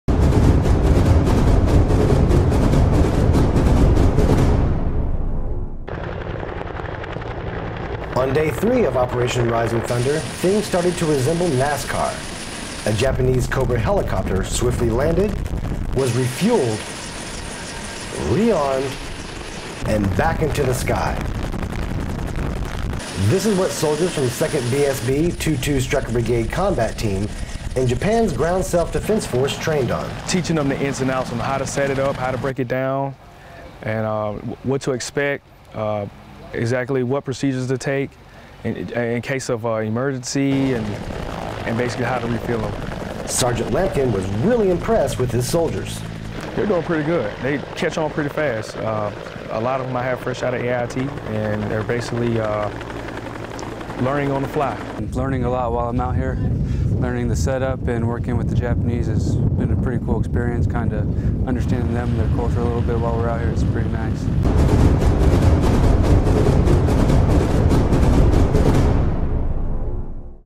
Rising Thunder Day 3
Soldiers from 2nd BSB, 2-2 SBCT and Japans Ground Self Defense Force work together refueling and rearming Japanese Cobra Helicopters.